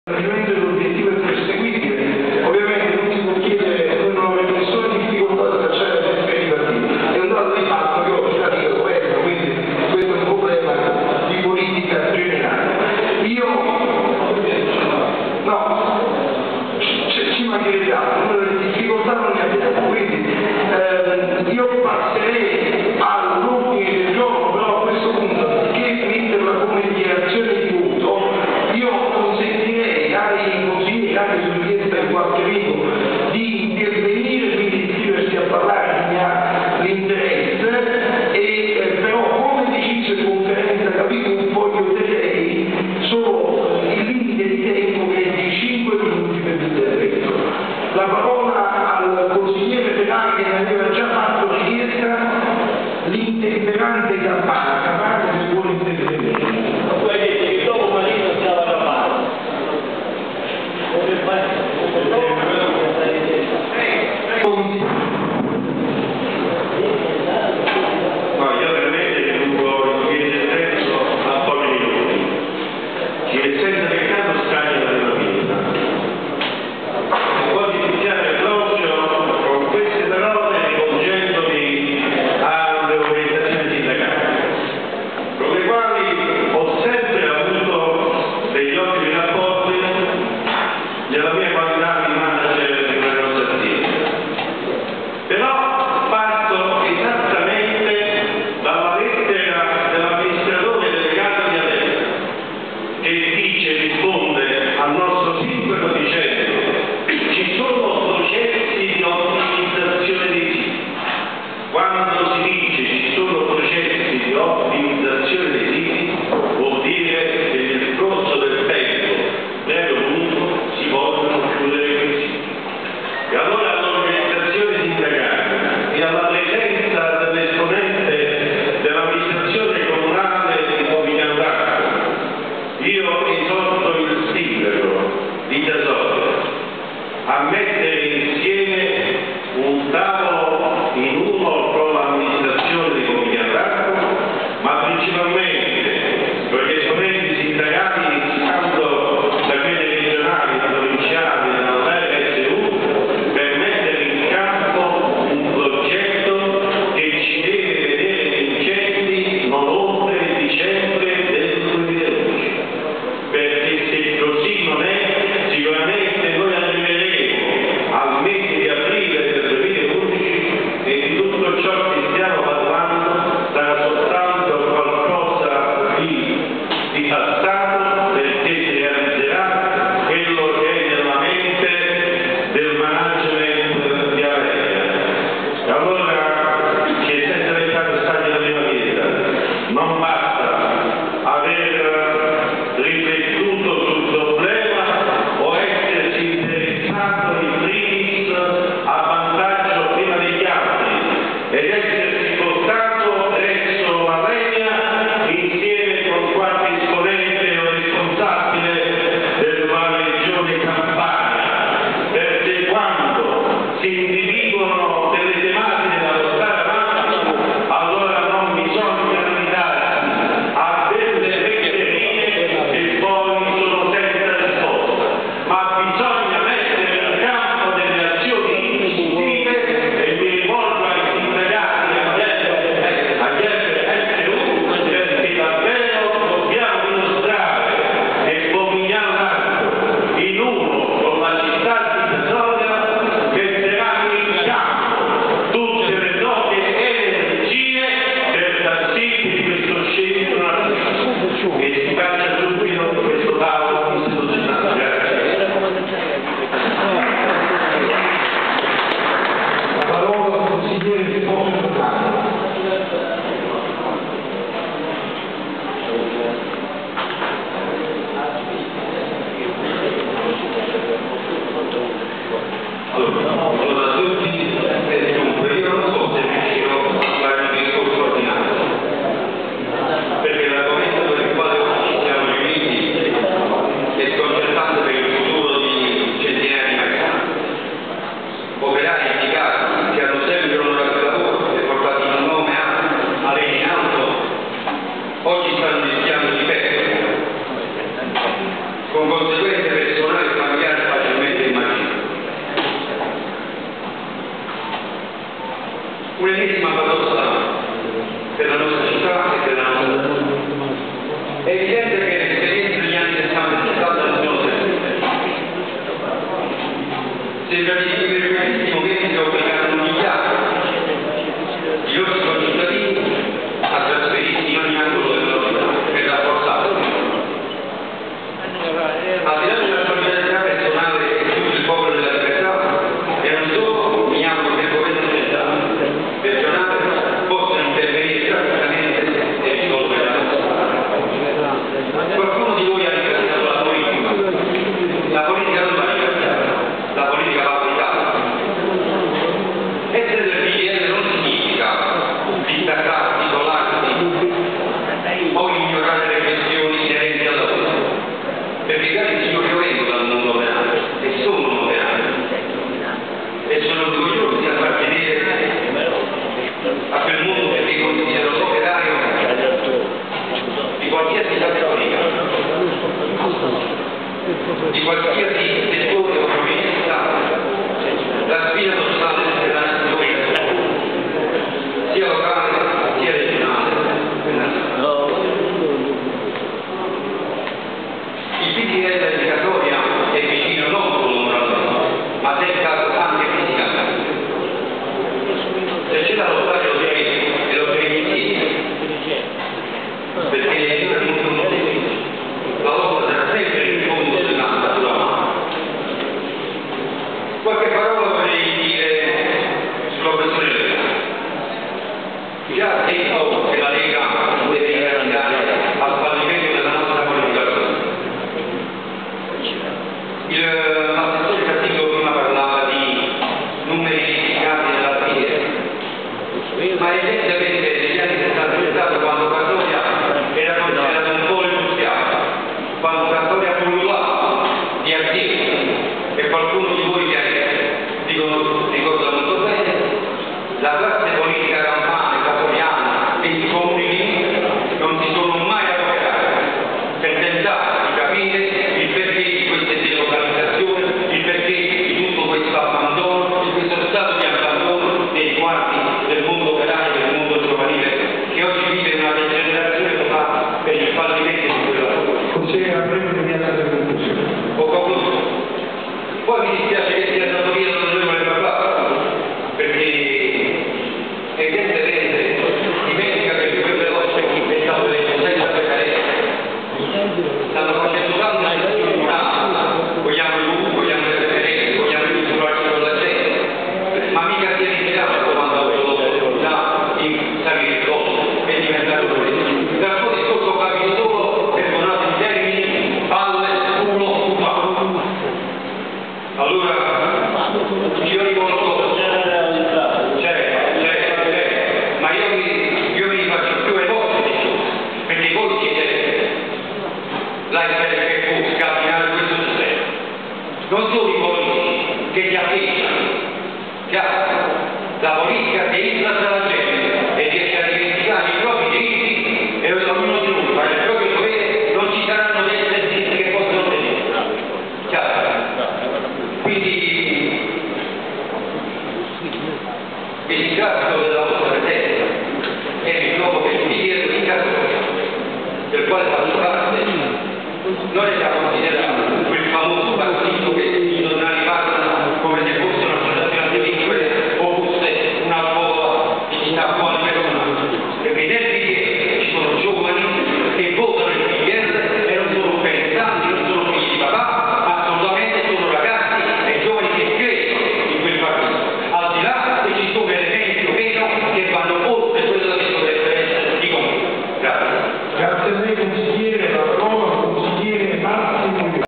CASORIA 26/092011 - C. C. INTERISTITUZIONALE "VICENDA ALENIA" (18)
"Casoria Ieri & Oggi online" presenta:
Lunedì 26 settembre 2011, con inizio alle ore 10:00, presso la Sala Consiliare del Comune di Casoria sita in piazza Cirillo.
Consiglio Comunale straordinario, aperto alle altre Istituzioni, per discutere sul salvataggio dello stabilimento Alenia di Casoria, destinato alla chiusura dal nuovo Piano industriale derivante dalla fusione di Alenia-Airmacchi con Alenia-Aeronautica.
Al Consiglio Comunale casoriano prenderanno parte i rappresentanti della Regione Campania, Provincia di Napoli e dei territori limitrofi, tra cui il Comune di Pomigliano e Nola.